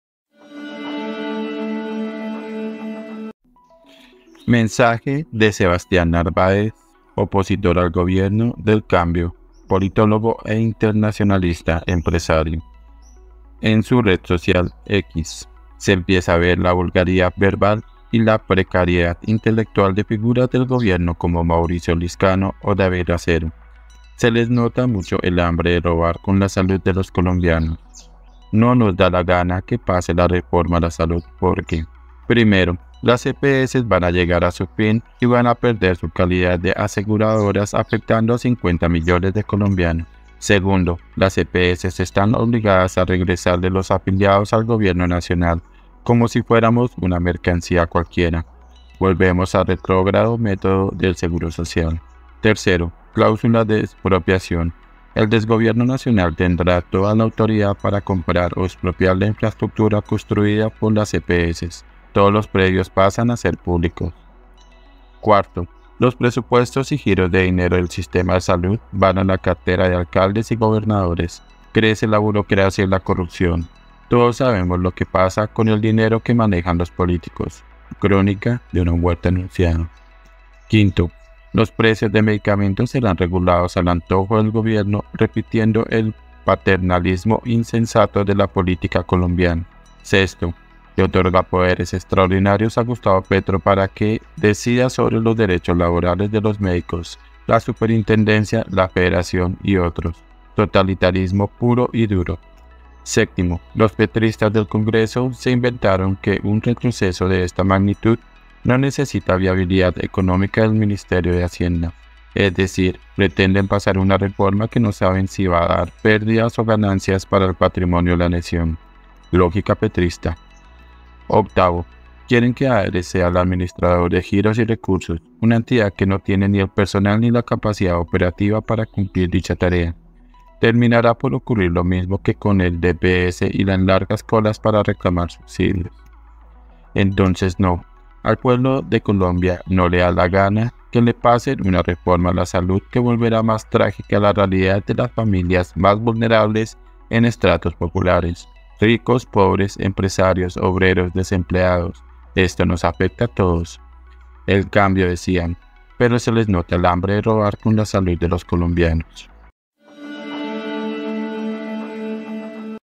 "LA VULGARIDAD VERBAL Y LA PRECARIEDAD INTELECTUAL DE DAVID RACERO Y MAURICIO LIZCANO": 📢 S Narvaez
Se empieza a ver la vulgaridad verbal y la precariedad intelectual de figuras del gobierno como Mauricio Lizcano o David Racero. Se les nota mucho el hambre de robar con la salud de los colombianos.
SEBASTIAN NARVAEZ